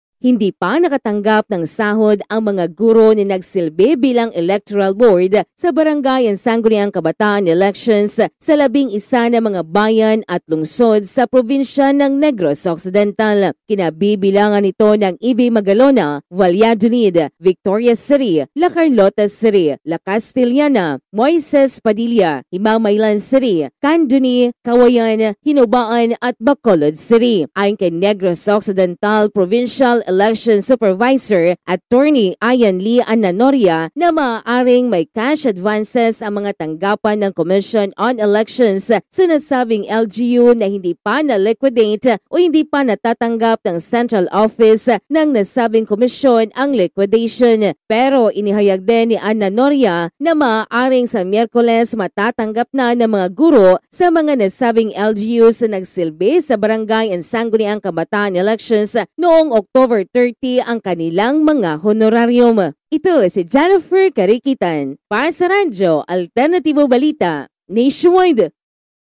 MGA GURO NA NAGSILBI SA BSKE SA 11 NA MGA LUGAR SA NEGROS OCCIDENTAL, HINDI PA NAKATANGGAP NG SAHOD
MGA GURO NA NAGSILBI BILANG ELECTORAL BOARD SA BSKE 2023 SA 11 NA MGA BAYAN AT LUNGSOD SA NEGROS OCCIDENTAL, HINDI PA NAKATANGGAP NG SAHOD

: Jennifer Carikitan